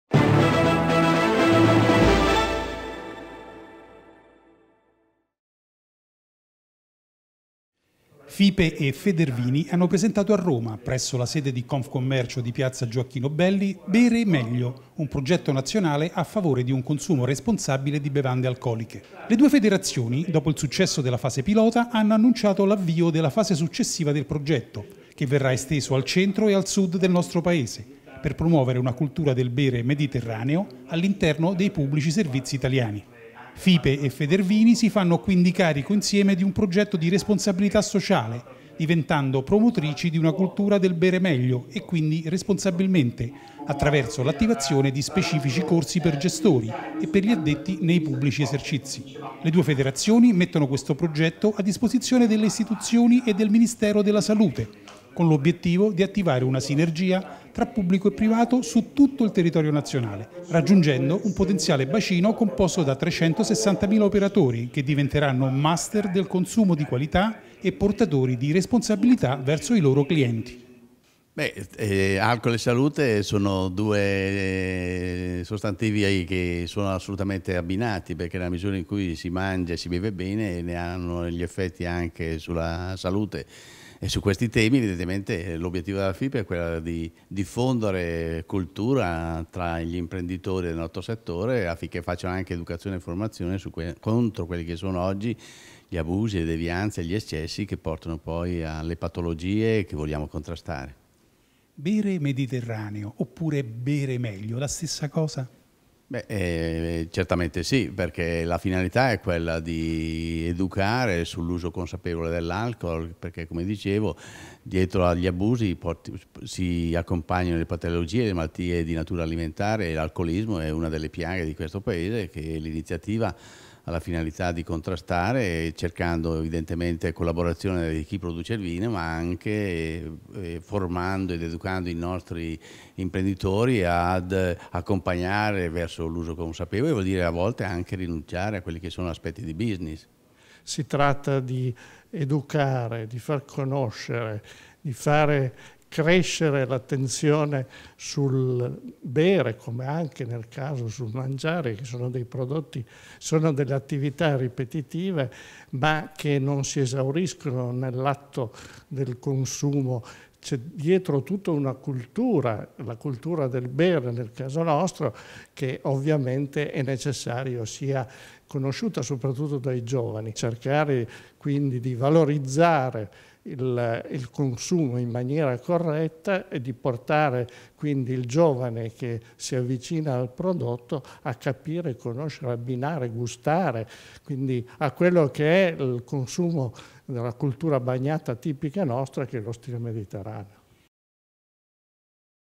Beremeglio: Consumo Responsabile è Consumo di Qualità
Il 27 giugno u.s. si è tenuta presso la Sala Cerroni della Fipe la conferenza stampa congiunta di Fipe e Federvini per annunciare, dopo il successo della fase pilota realizzata a Padova con la collaborazione della APPE, l’avvio della fase successiva del progetto, che verrà esteso al Centro e al Sud del Paese per promuovere una cultura del “bere mediterraneo” all’interno dei pubblici esercizi italiani e formare gli addetti al servizio di bevande alcoliche.